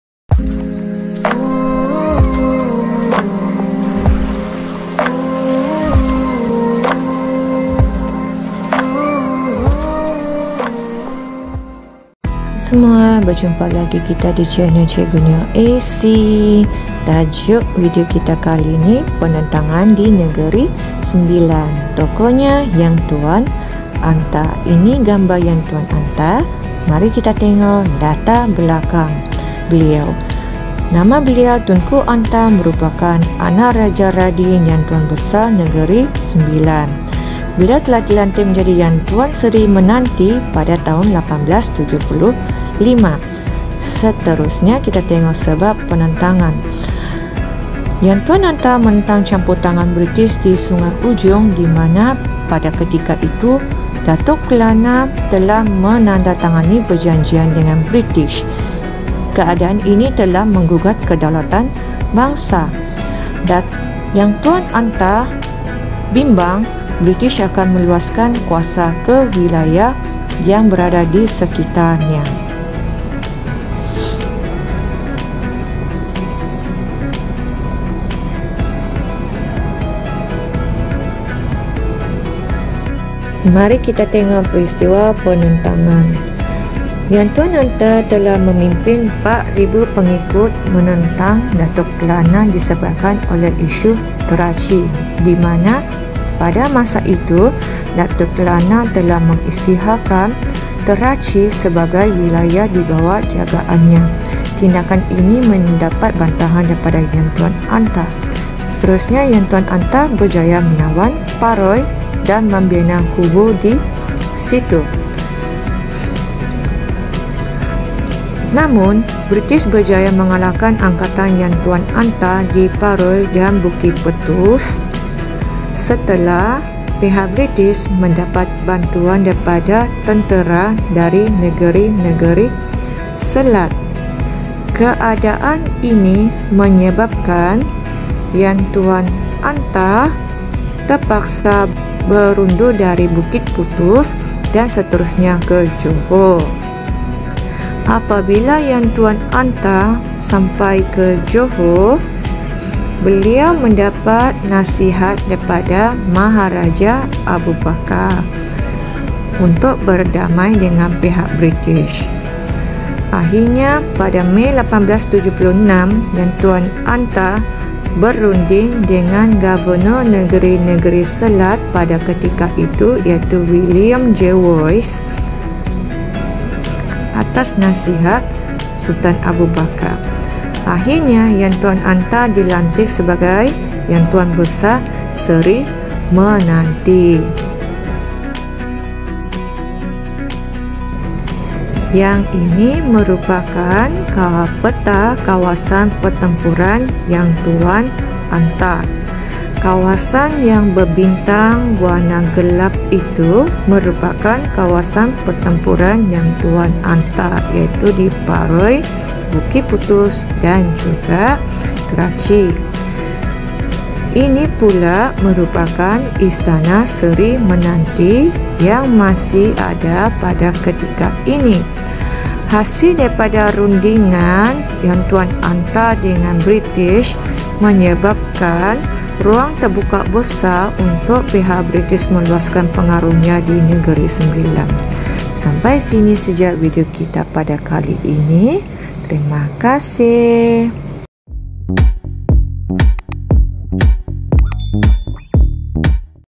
Sejarah Tingkatan 3 | Bab 7.1 | Matlamat dan Bentuk Penentangan Masyarakat Tempatan | Yamtuan Antah
Penentangan Yamtuan Antah di Negeri Sembilan
Bab 7.1 Matlamat dan bentuk penentangan masyarakat tempatan
Tingkatan 3 KSSM

Yamtuan Antah merupakan pemerintah yang mengetuai penentangan di Negeri Sembilan.

Bentuk penentangan Yamtuan Antah adalah dalam bentuk penentangan bersenjata.

Campur tangan kuasa Barat di negara kita membawa perubahan kepada sistem pentadbiran tempatan.

Perubahan tersebut menyebabkan masyarakat tempatan tidak berpuas hati kerana kehilangan hak dan kedudukan mereka.

Oleh itu, mereka bangkit menentang kuasa British di negara kita melalui penentangan bersenjata, mencabar perjanjian dan sistem perundangan.

Penentangan masyarakat tempatan terhadap British diketuai oleh golongan bangsawan tempatan, pembesar-pembesar tempatan dan pemimpin agama yang tidak berpuas hati dengan pembaharuan yang dibuat oleh British